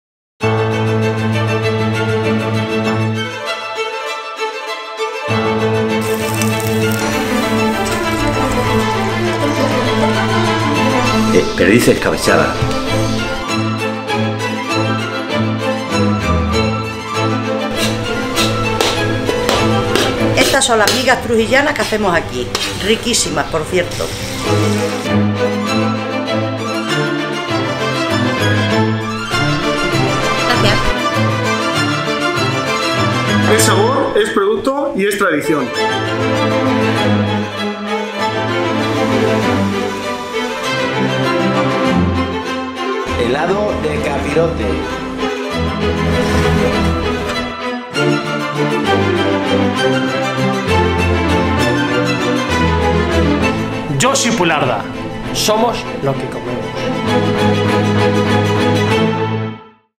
CÁCERES TOLEDO - LA VUELTA GASTRONÓMICA 2022
Video promocional RUTA GASTRONÓMICA LA VUELTA 2022 por BURGOS ALIMENTA